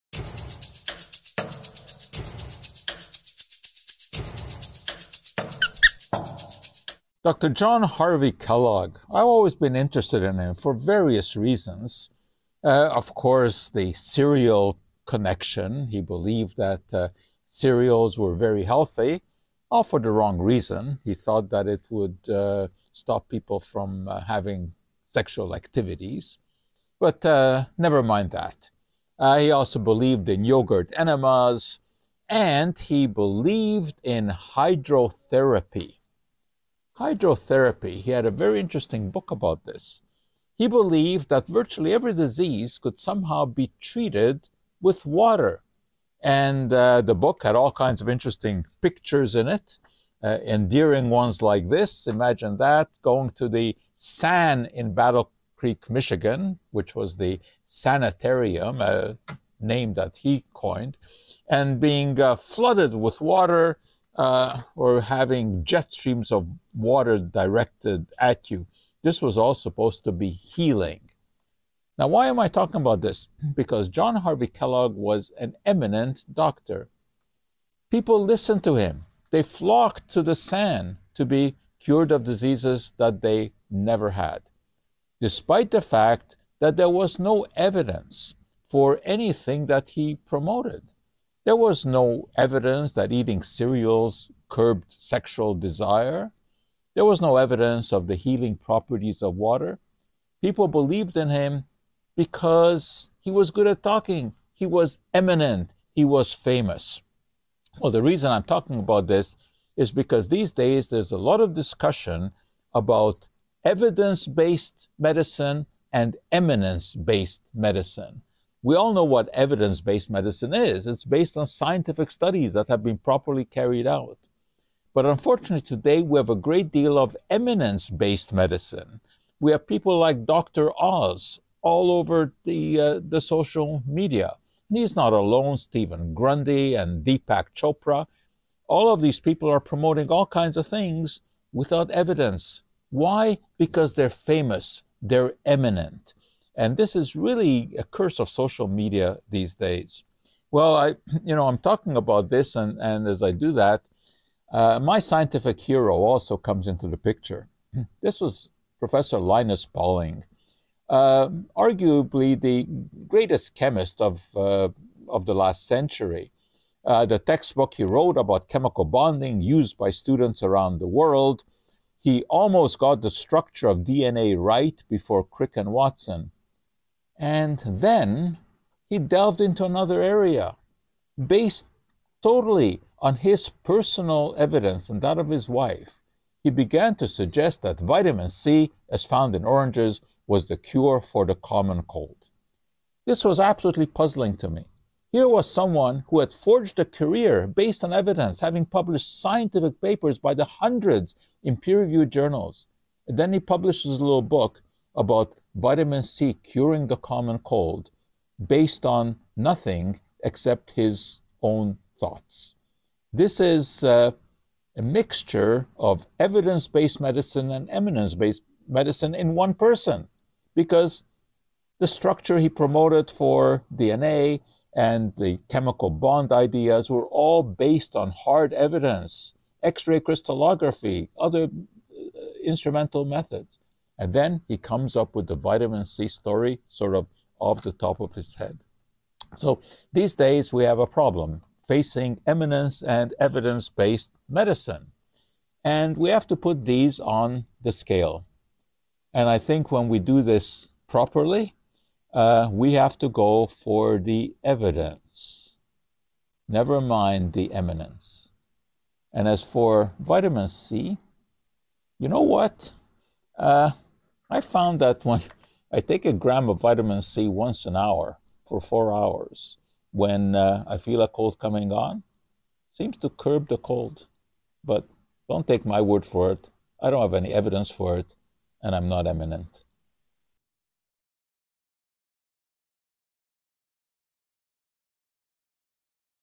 The Right Chemistry-To Whom Do We Listen for Advice?
There is a difference between Evidence-based and Eminence-based medicine